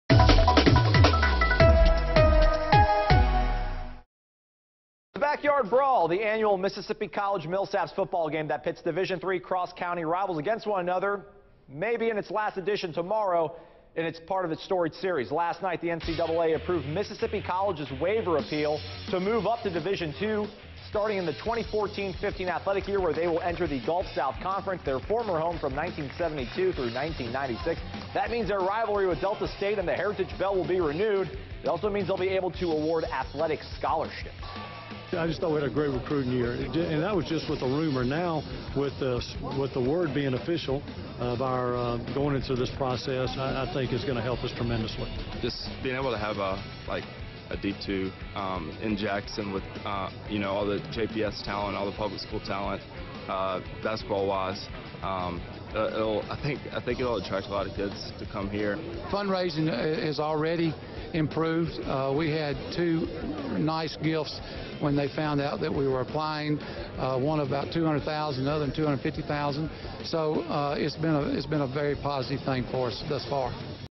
Mississippi College to move up to NCAA Division II athletics
Mississippi College to move up to NCAA Division II athletics and play in the Gulf South Conference.